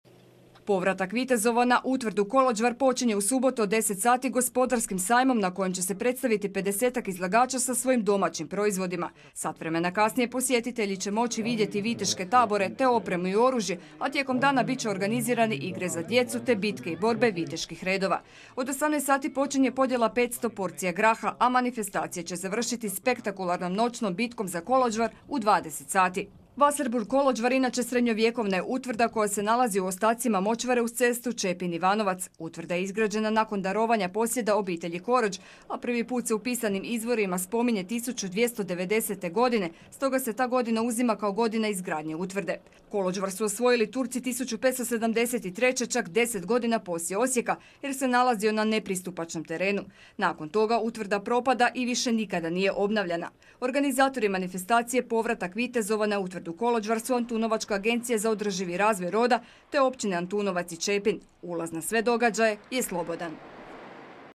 Vitezovi se u subotu vraćaju na utvrdu Kolođvar
Srednji vijek obilježili su vitezovi – hrabri borci u metalnim oklopima s mačevima i štitovima. A kako su se s tako teškom opremom borili moći će vidjeti svi koji u subotu posjete manifestaciju Povratak vitezova na utvrdu Kolođvar 2016.
Ovaj povijesni lokalitet smješten je između Ivanovca i Čepina, te je registriran kao kulturno dobro Republike Hrvatske.